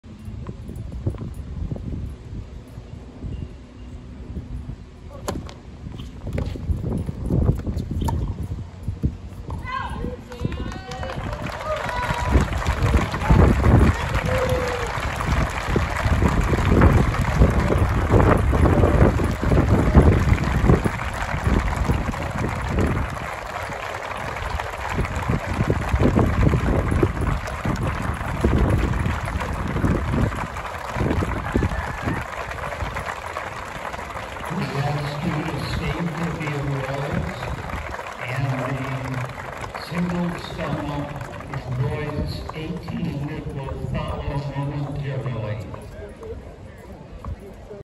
Match point, USTA Boys National 16s Championship, August 11, 2024, Stowe Stadium Kalamazoo College
Gus Grumet defeated Arin Pallegar 6-7(4), 6-2, 7-6(2).